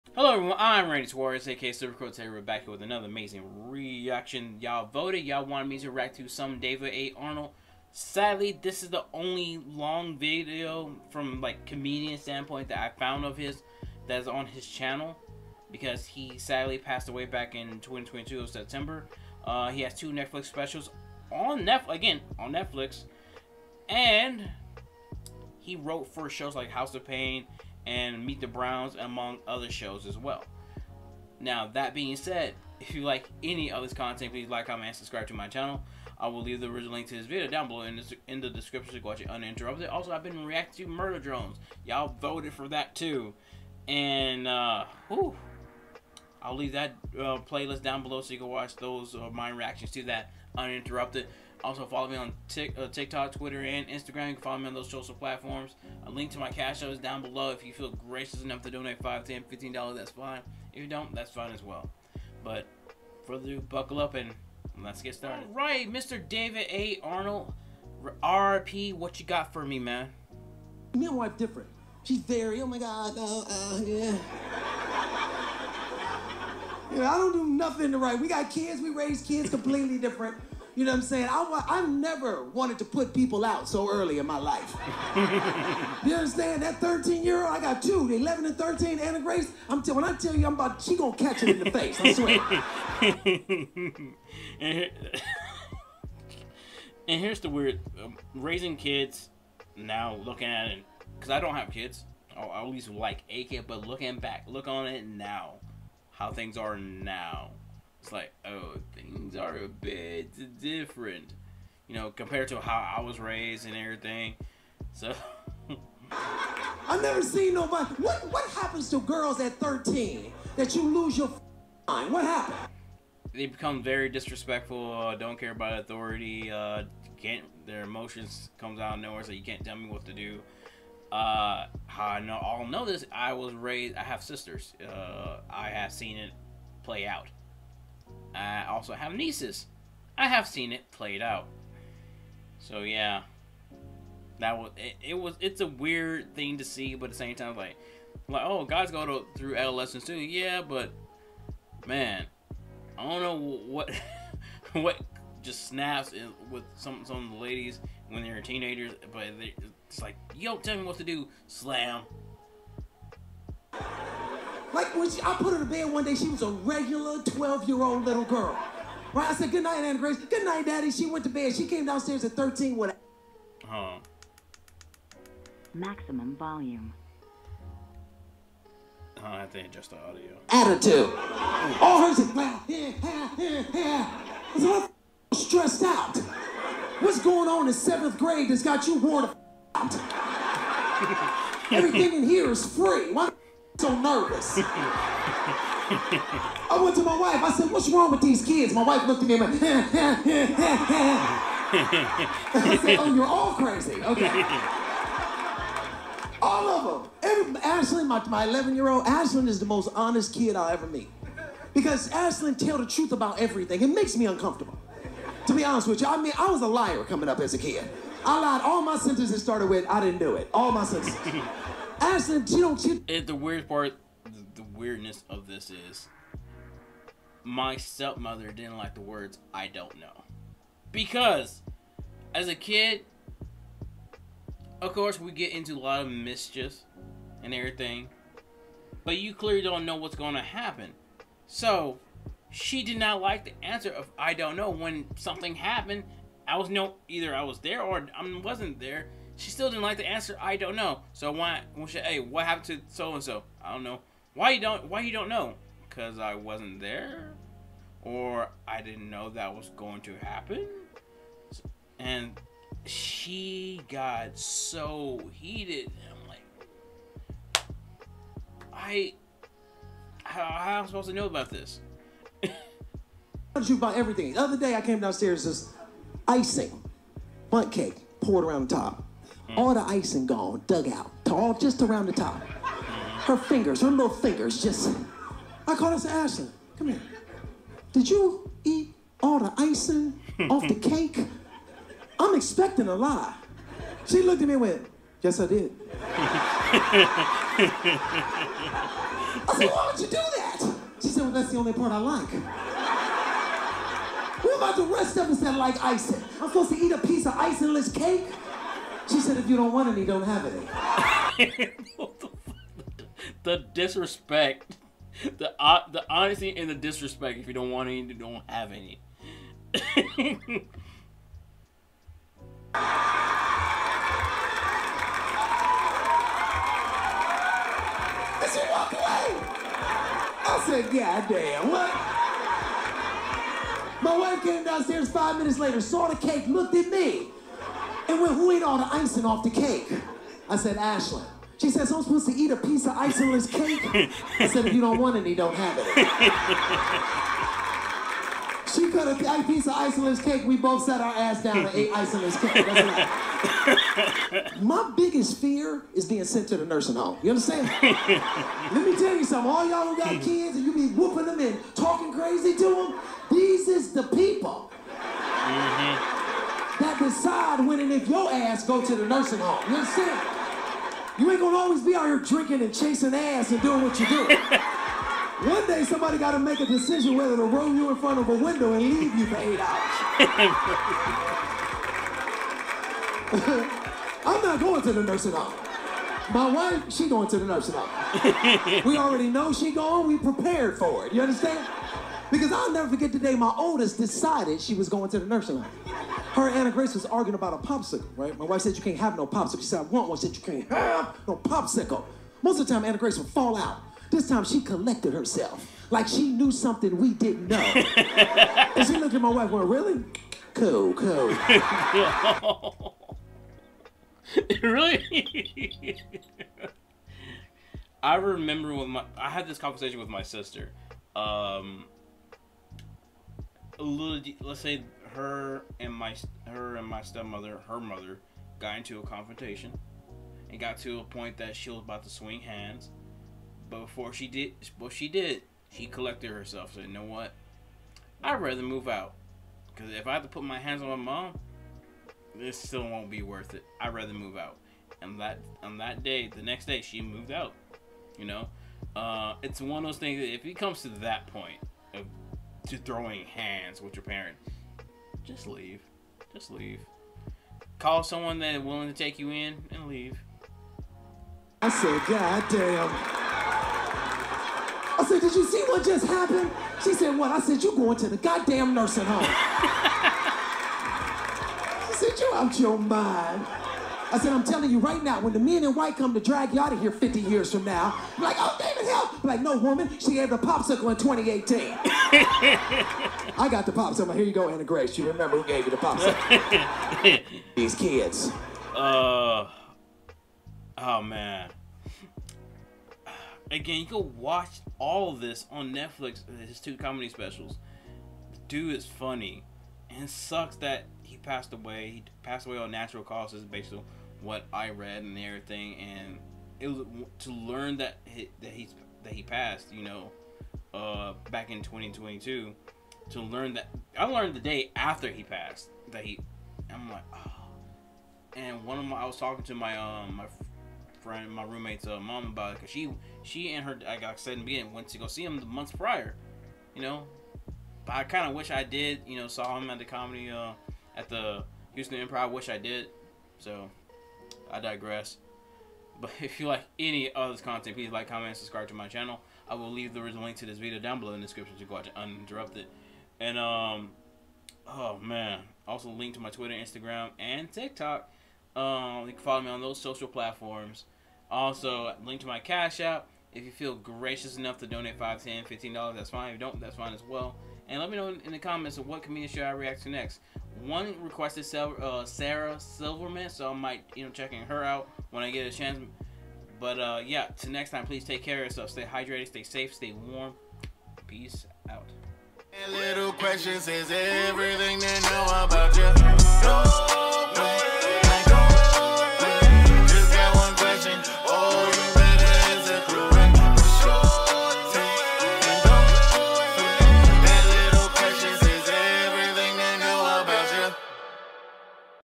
David A. Arnold - Me and My Wife Are Different Kinds of Parents - Silva Crow Reacts
R.I.P. to @DavidAArnold
Very Funny comedian & a awesome human being.